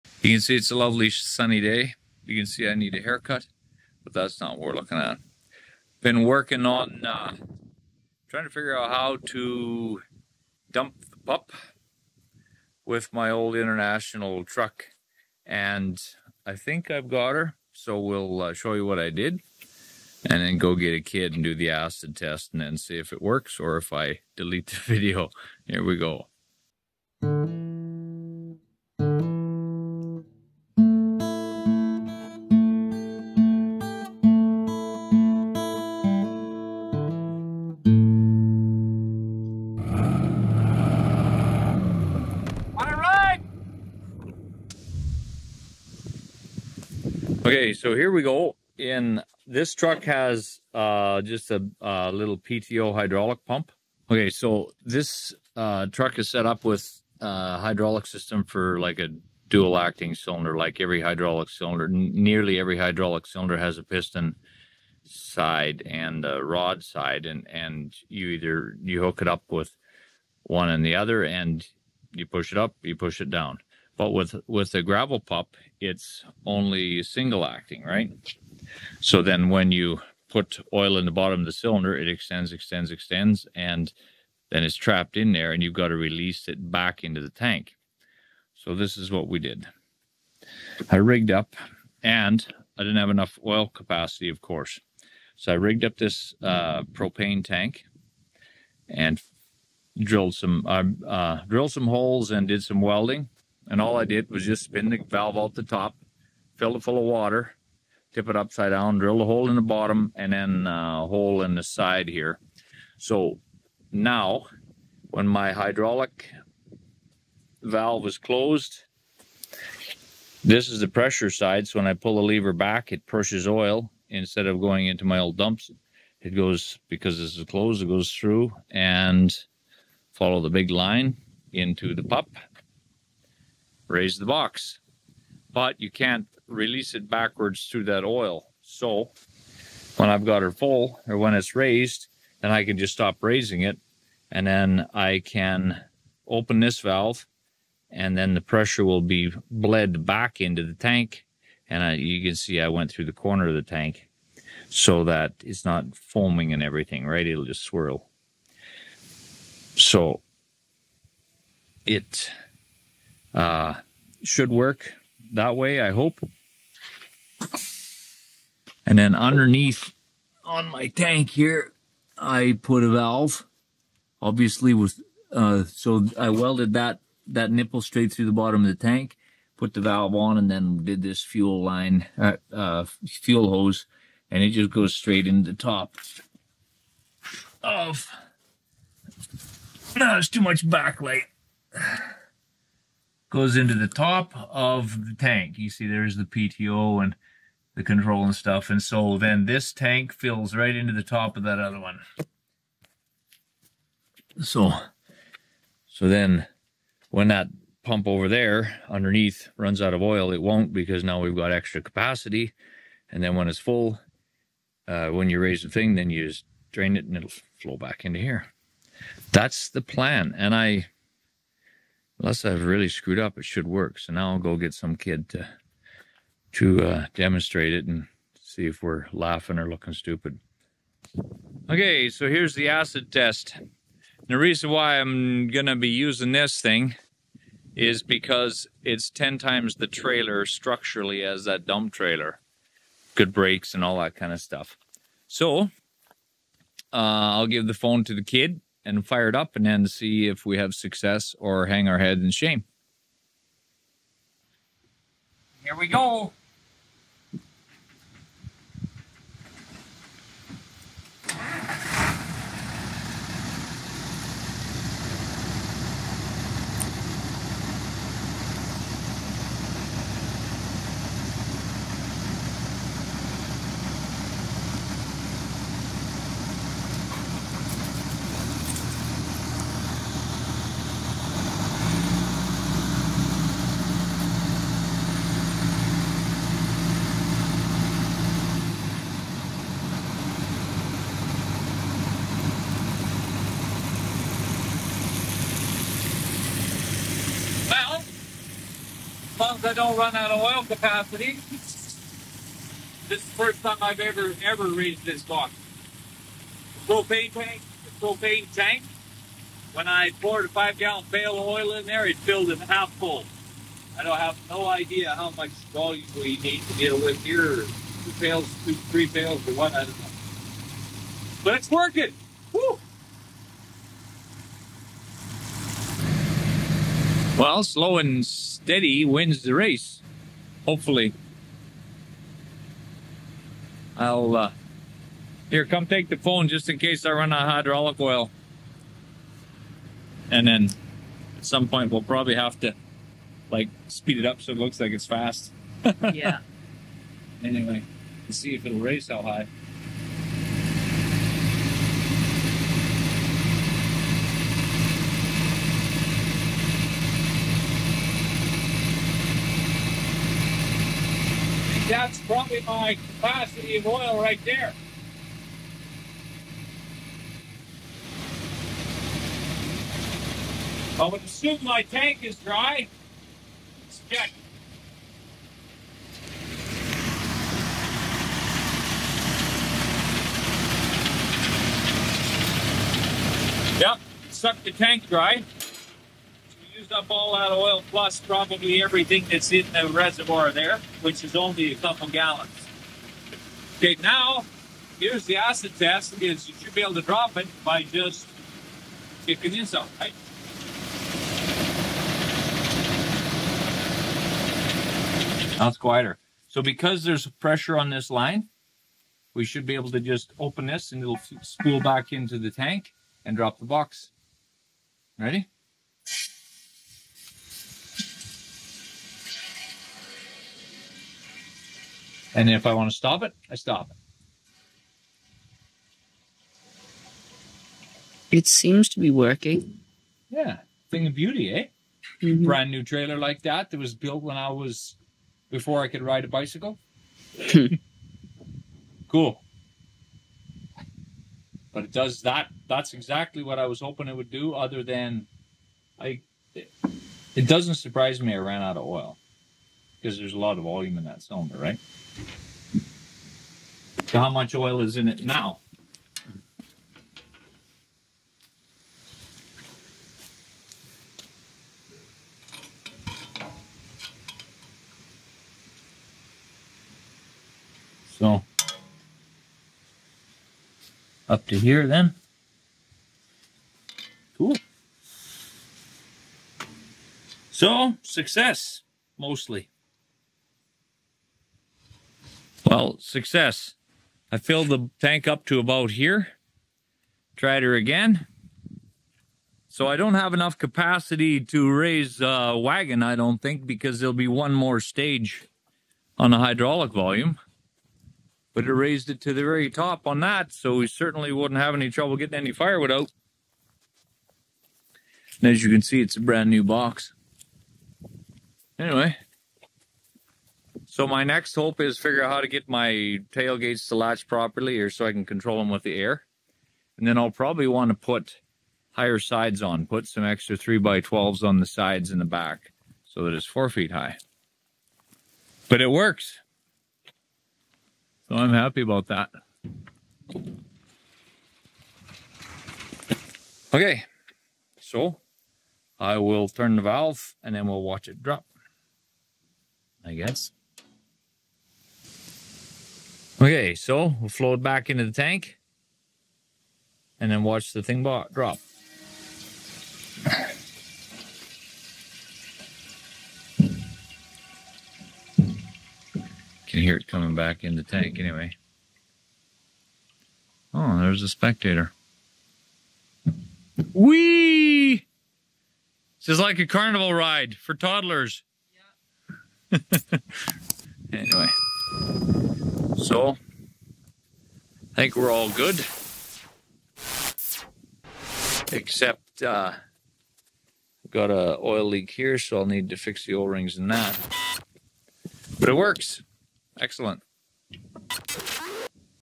DIY increasing hydraulic capacity
DIY increasing hydraulic capacity. I needed to increase capacity for my old trucks hydraulic system so that I could raise the box on my pup trailer. I up-cycled an old propane tank and some hoses to get the job done.
The real question was trying to run a single acting hydraulic cylinder with a dual acting valve. Generally the oil is pushed into the barrel on one side of the piston while being taken out of the other side so the oil capacity doesn't have to be very much. With a multi stage single acting like on the pup, it just pushes oil into the cylinder which extends the stages, taking much more volume. The other problem is getting the oil to release. It seems like it works ok. Slow, but functional.